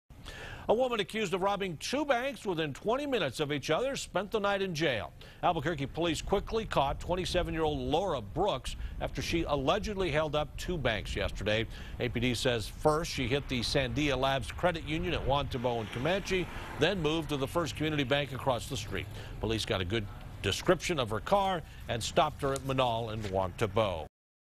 Police ID woman accused in bank heists
The woman accused of robbing two Albuquerque banks within 20 minutes of each other spent the night in jail.